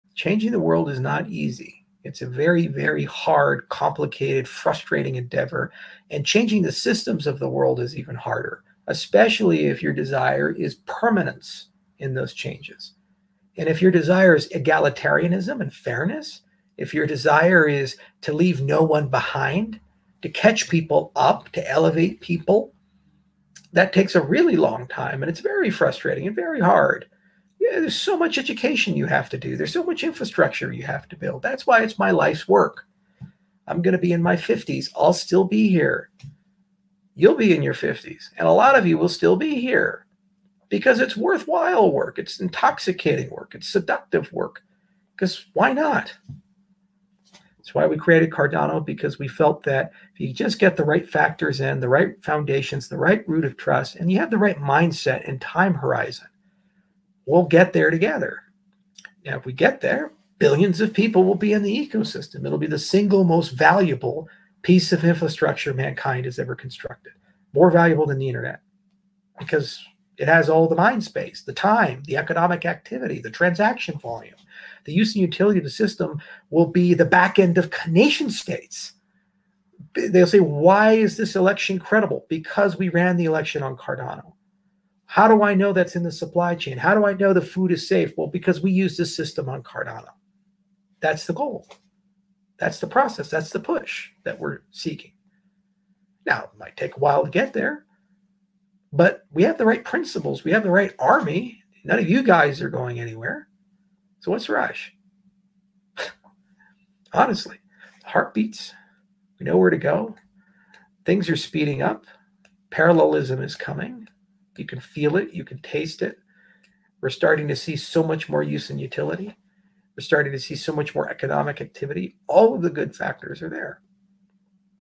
Charles Hoskinson - Changing the World
Clip taken from Charles Hoskinson's Musings and a bit of an AMA (Back from Africa) 11/13/2021.